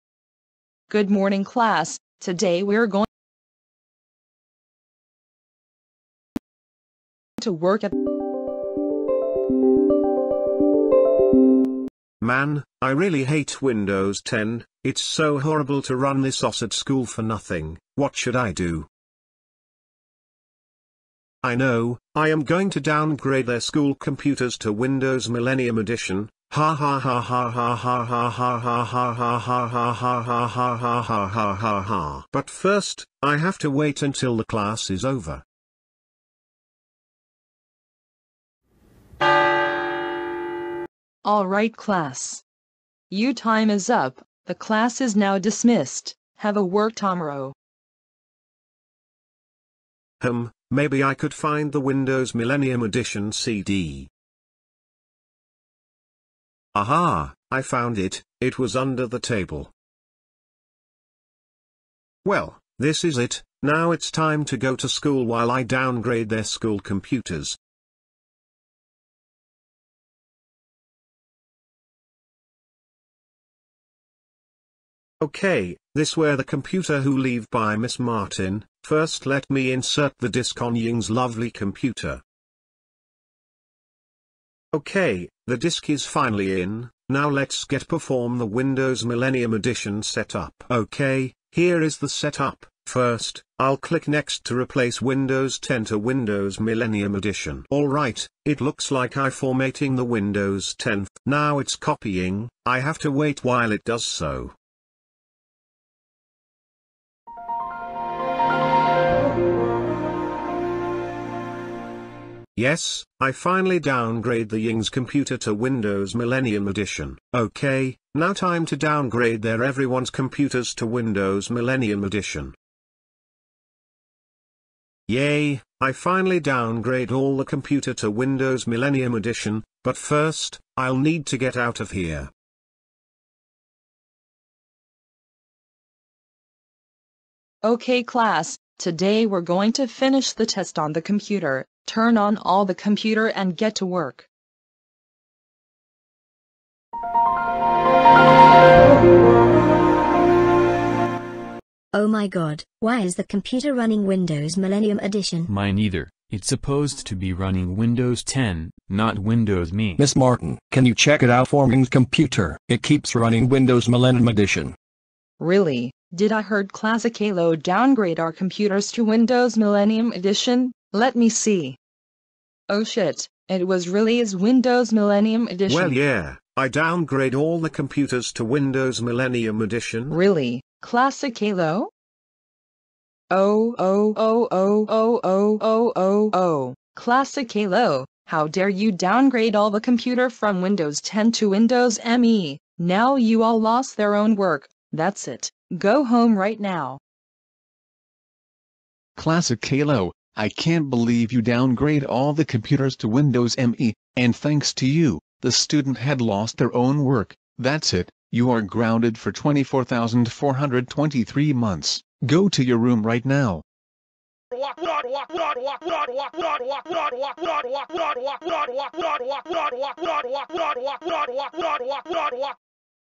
Classic Caillou Downgrade All School Computer from Windows 10 to Windows Millennium Edition/Grounded
Oh, Classic Caillou downgrades all the computer from windows 10 to windows me, nothing else but it's bad to downgrade it.